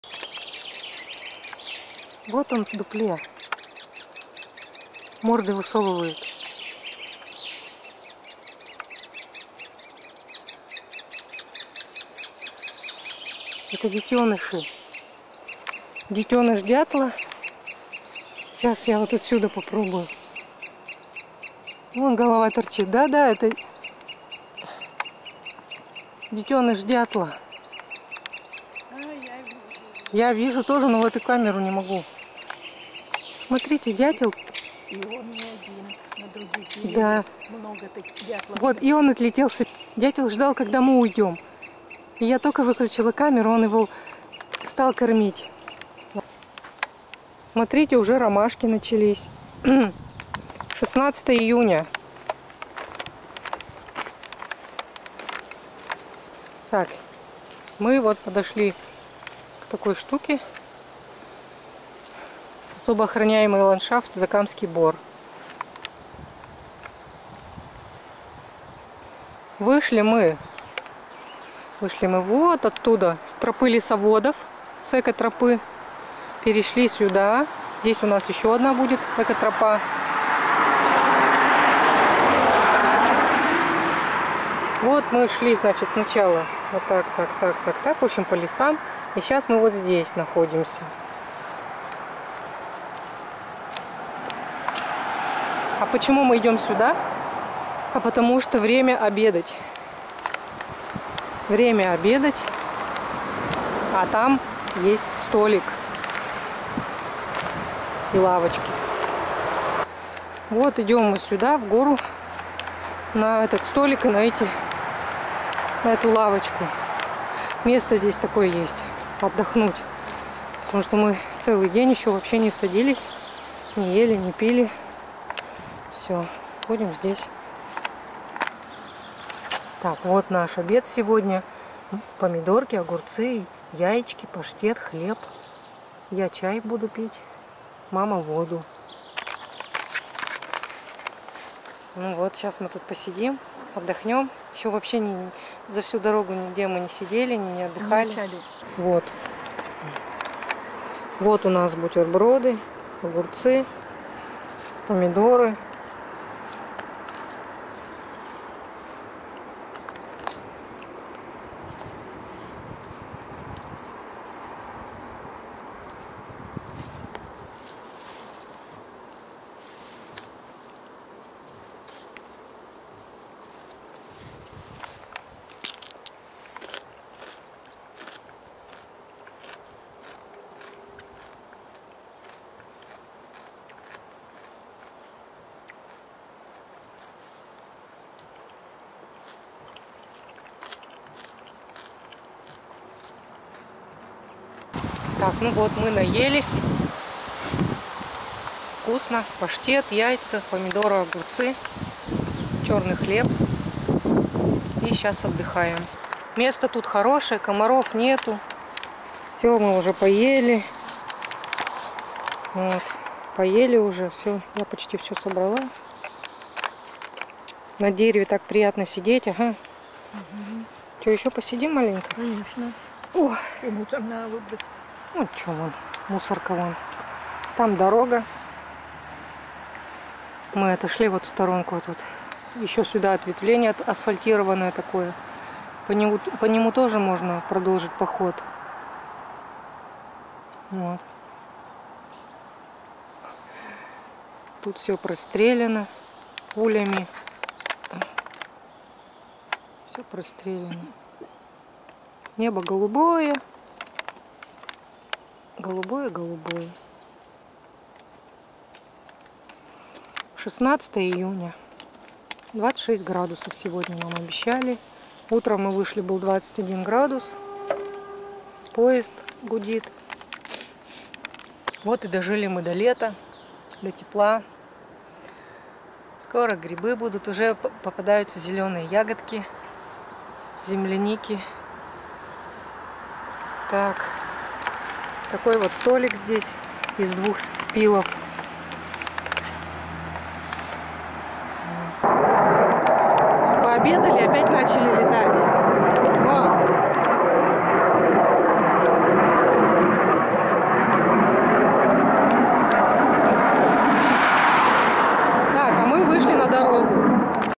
Обед в лесу и Дятлы на гнезде 16 июня 2020
Лето. Верхняя Курья. + 26 градусов. 16 июня 2020
Шли по сосновому лесу и долго слышали чей-то пронзительный писк. Оказалось, что это птенцы дятла пищат из дупла. Середина июня это время подрастания молодых дятлов. Это большой дятел - его можно узнать по красной шапке.